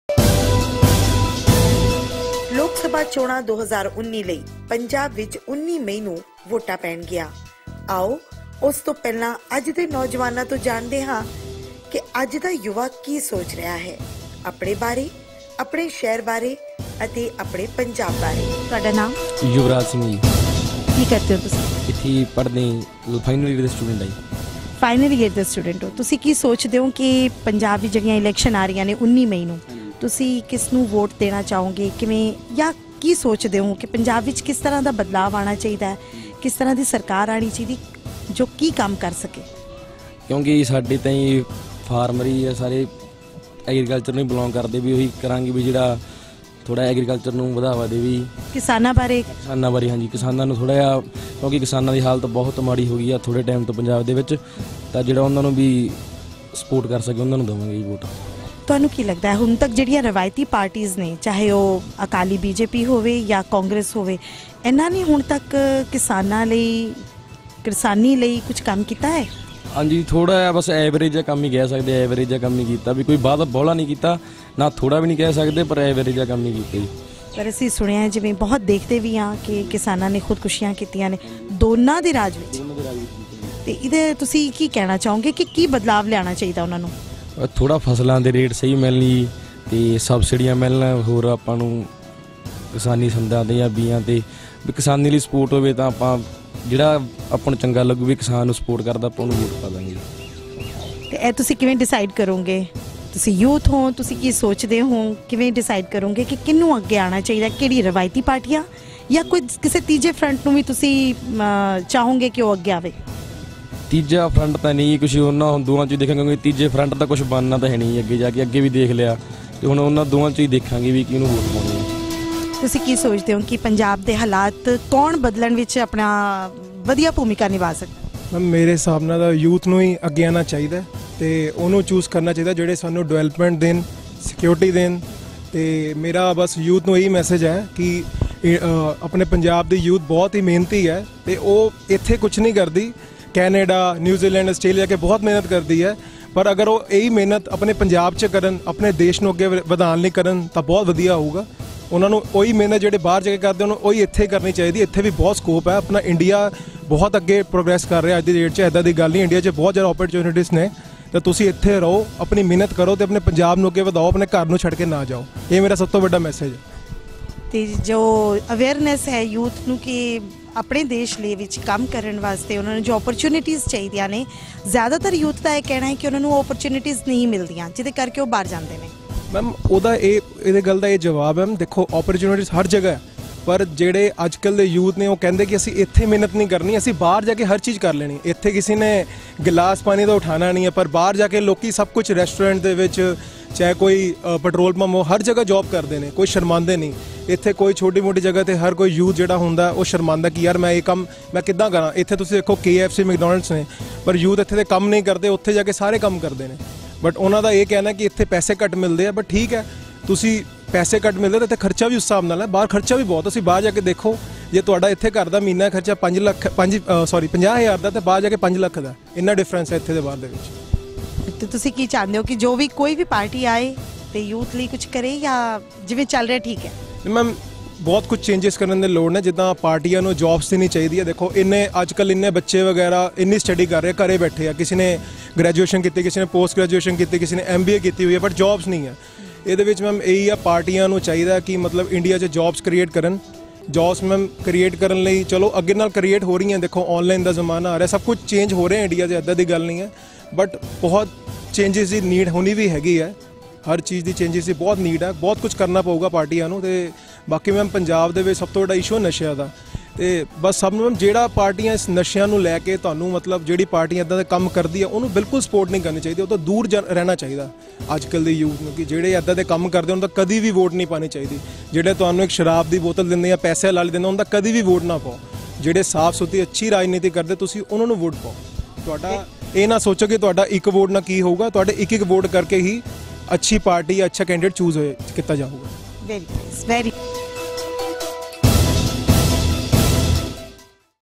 Youth Interview Ludhiana
ਅੱਜ ਦਾ ਯੁਵਾ ਕੀ ਸੋਚ ਰਿਹਾ ਹੈ! ਆਪਣੇ ਬਾਰੇ, ਆਪਣੇ ਸ਼ਹਿਰ ਬਾਰੇ ਅਤੇ ਆਪਣੇ ਪੰਜਾਬ ਬਾਰੇ
What's in MIND of Today's YOUTH! about His, his City and his